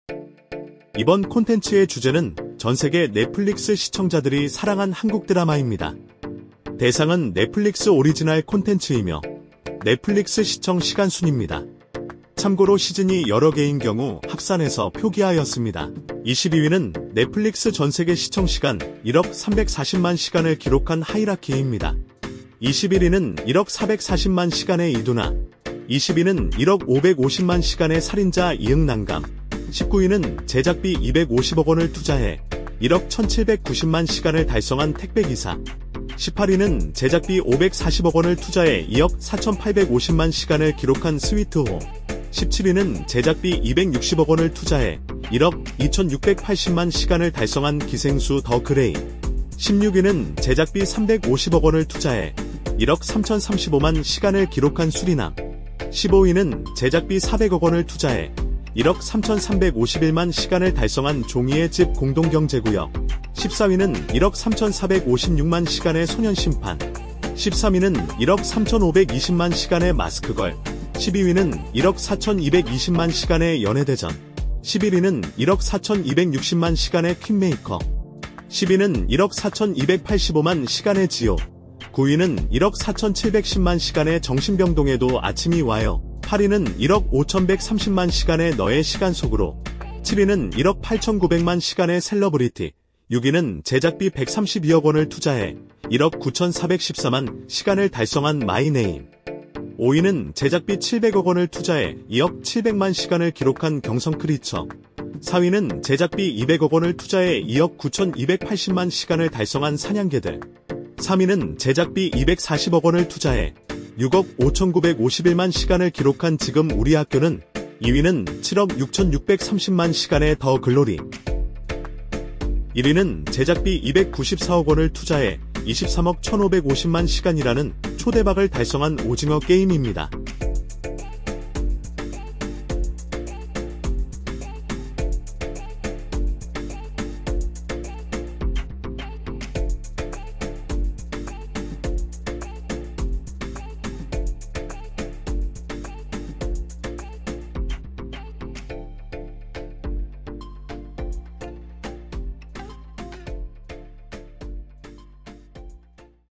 0922 🎞넷플릭스 1억 시청시간을 돌파한 국내 드라마(오리지널 편) | 오징어게임,더글로리,지우학,사냥개들,경성크리처,마이네임,셀러브리티
🎞넷플릭스 1억 시청시간을 돌파한 국내 오리지널 드라마
1. 오징어 게임  23억 1,550만 시간 (제작비 294억원)
2. 더 글로리  7억 6,630만 시간
3. 지금 우리 학교는  6억 5,951만 시간 (제작비 240억원)
4. 사냥개들  2억 9,280만 시간 (제작비 200억원)
5. 경성크리처  2억 700만 시간 (제작비 700억원)
6. 마이 네임  1억 9,414만 시간 (제작비 132억원)
7. 셀러브리티  1억 8,900만 시간
8. 너의 시간 속으로  1억 5,130만 시간
9. 정신병동에도 아침이 와요  1억 4,710만 시간
10. 지옥  1억 4,285만 시간
11. 퀸메이커  1억 4,260만 시간
12. 연애대전  1억 4,220만 시간
13. 마스크걸  1억 3,520만 시간
14. 소년심판  1억 3,456만 시간
15. 종이의 집: 공동경제구역  1억 3,351만 시간 (제작비 400억원)
16. 수리남  1억 3,035만 시간 (제작비 350억원)
17. 기생수: 더 그레이  1억 2,680만 시간 (제작비 260억원)
18. 스위트홈 2억 4,850만 시간 (제작비 540억원)
19. 택배기사  1억 1,790만 시간 (제작비 250억원)
20. 살인자ㅇ난감  1억 550만 시간
21. 이두나!  1억 440만 시간
22. 하이라키  1억 340만 시간

*출처 : 넷플릭스 외 종합
*대상 : 넷플릭스 전세계 1억 시청시간을 돌파한 국내 오리지널 드라마
*단위 : 시청시간(H)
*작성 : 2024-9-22
*넷플릭스 누적 시청시간은 매주 공개하는 부문별 TOP10과 반기별로 공개하는 시청시간을 종합하여 적용하였습니다.